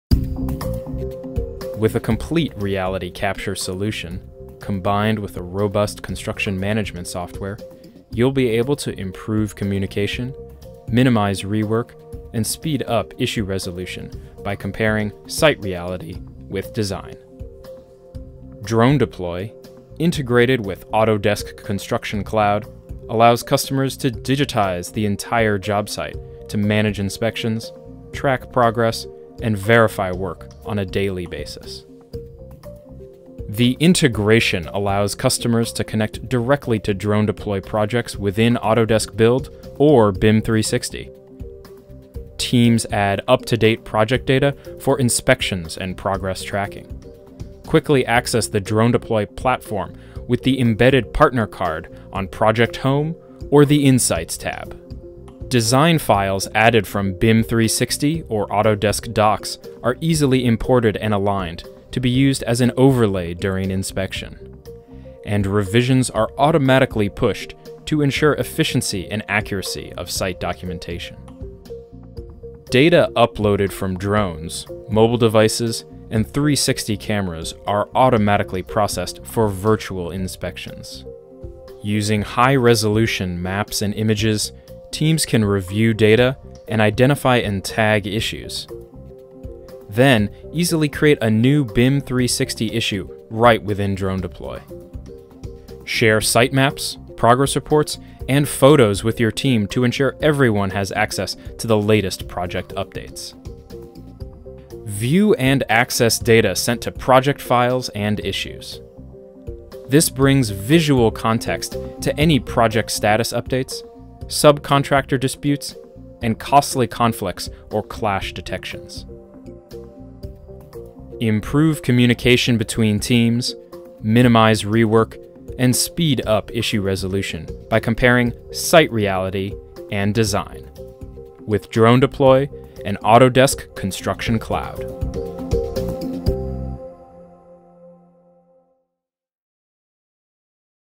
Streamline Inspection Workflows: Connect BIM 360 With DroneDeploy
Improve communication between teams, minimize rework, and speed up issue resolution by comparing site reality and design with DroneDeploy and the Autodesk Construction Cloud.